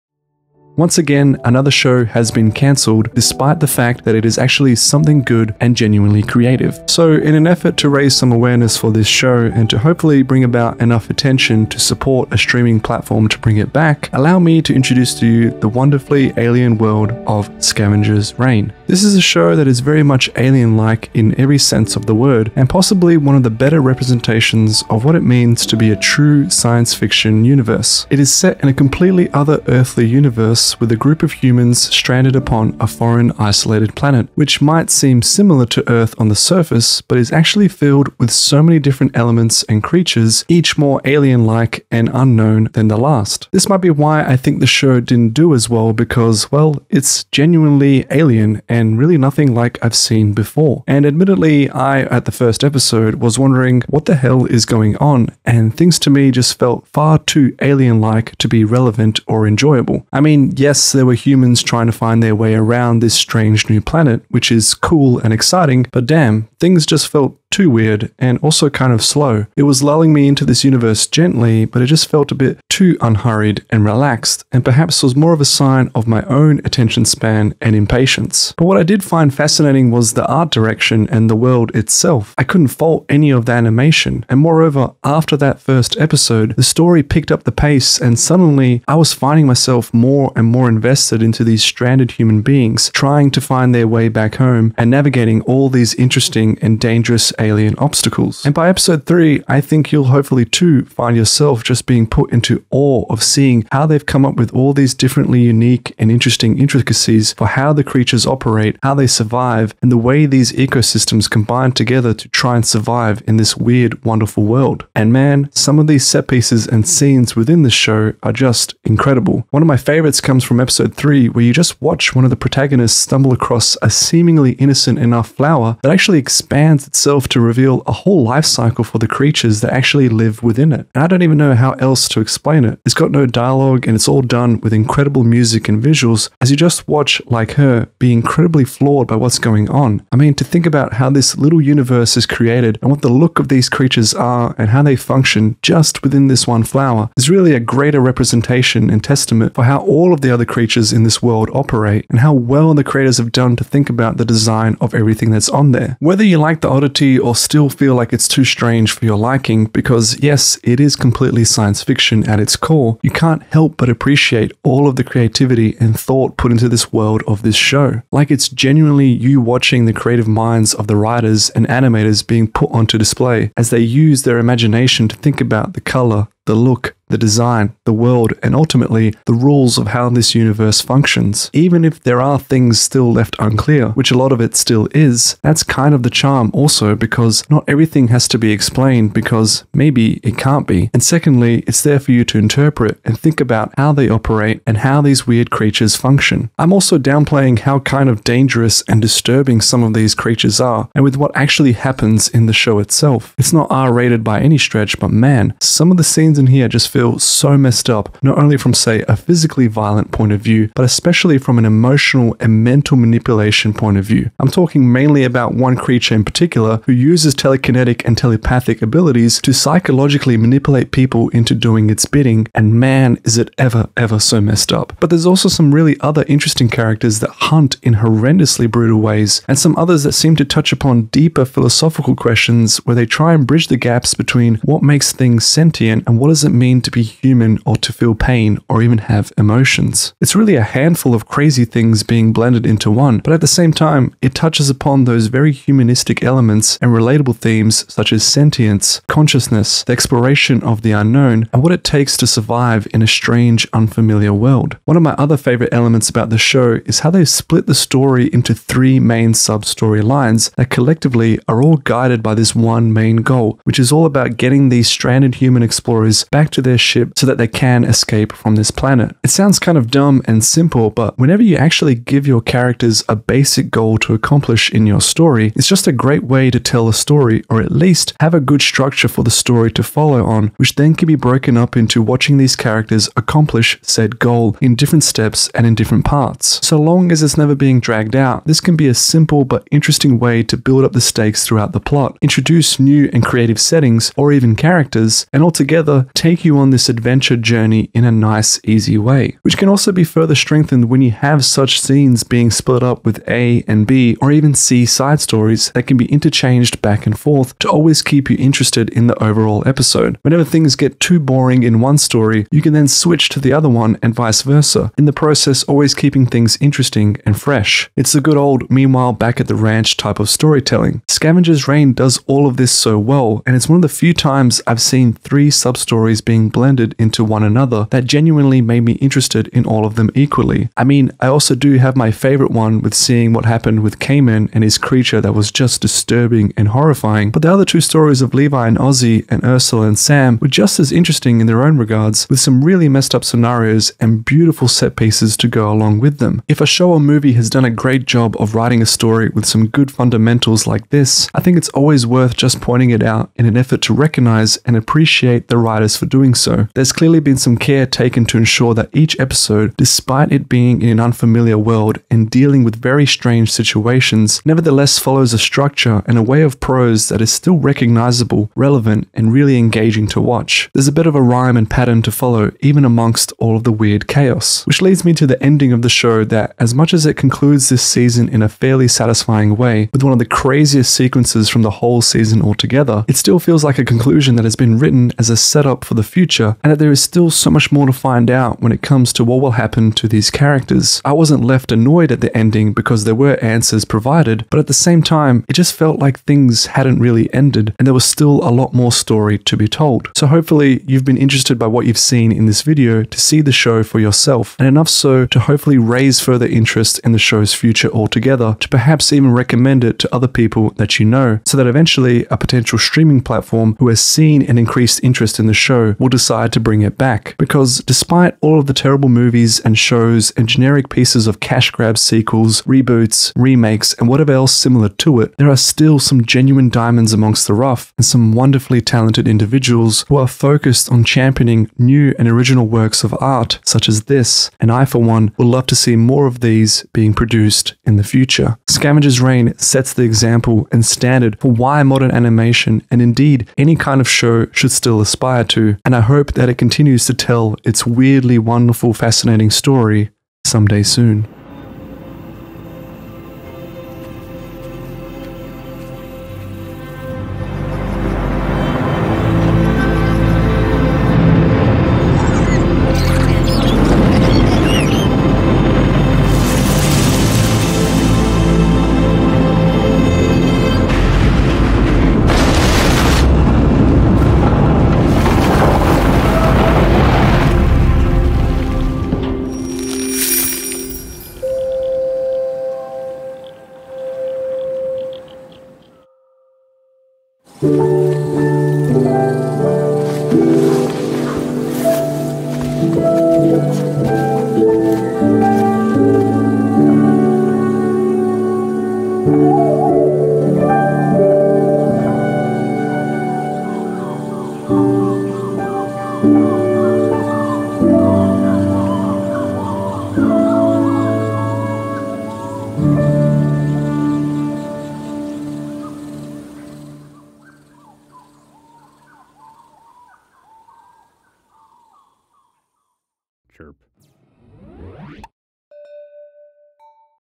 Gone Too Soon - The Tragedy of Scavengers Reign
Once again, another show has been cancelled even though it is good and creative. So, to raise some awareness for this show and hopefully bring about enough attention to support a streaming platform bringing it back, allow me to introduce you to the wonderfully alien world of Scavengers Reign.

These include the lovely people “Matthew McKinley", "Adam McCarter", "FloNess" and "ThatEvilCanadian” at the time this was made.

What is Scavengers Reign?

Scavengers Reign is an American adult animated science fiction drama television series created by Joseph Bennett and Charles Huettner for Max, based on their 2016 short film Scavengers. HBO Max announced the series in June 2022, and it premiered on October 19, 2023. Since the show's release, it has received critical acclaim, praising the acting, plot, animation, and atmosphere.

In May 2024, the series was canceled after one season.

The series follows the survivors of the damaged interstellar cargo ship Demeter 227 who are stranded on Vesta, an alien planet bustling with flora and fauna but filled with dangers. Initially, the survivors are separated into three groups: Azi and her robot companion Levi, Sam and Ursula, and the isolated Kamen, who travels with a telepathic creature named Hollow. Over time, all their paths converge towards the crash-landed Demeter.

Joseph Bennett and Charles Huettner created the original 2016 Scavengers short film for Adult Swim, where it aired as part of the Toonami block. The eight-minute animated short contains no dialog and depicts a pair of humans engaging in an intricate series of interactions with alien creatures. From 2018 to 2019, Bennett and Huettner expanded the concept into a series pilot that did not go forward at Adult Swim, though the show was later picked up by HBO Max. The twelve-episode series was produced over two years by Titmouse, Inc. and Green Street Pictures with a crew including international animators working remotely from locations including Mexico, Spain, Portugal, and France.

In May 2024, the series was canceled by Max. However, Netflix acquired the first season and was reported to be considering renewing the series with a decision to be made after its debut on the platform. In November 2024, Bennett confirmed that Netflix had decided not to renew the series for a second season.

The series premiered on October 19, 2023, with three episodes released weekly until November 9, 2023. In May 2024, the series was acquired by Netflix and released on May 31 in the United States, United Kingdom, Ireland, and New Zealand.

The series has received critical acclaim. On review aggregator website Rotten Tomatoes, the first season has a 100% approval score with an average rating of 8.7/10, based on 22 critic reviews. The website's critics consensus reads, "Disturbing and wondrous, Scavengers Reign presents a vividly realized world that beckons exploration by its marooned characters and television viewers alike." James Poniewozik of The New York Times called it "a lush, magnificent, hypnotic story of human survival in a place that feels, in a way that sci-fi planets only occasionally manage, truly otherworldly." Andrew Webster of The Verge deemed it to be "equal parts beautiful and brutal, and it might be the most original piece of science fiction of the year."

For the 76th Primetime Creative Arts Emmy Awards, the series was nominated for Outstanding Animated Program and won an Outstanding Individual Achievement in Animation for background design.

And if you’re still reading this – hello.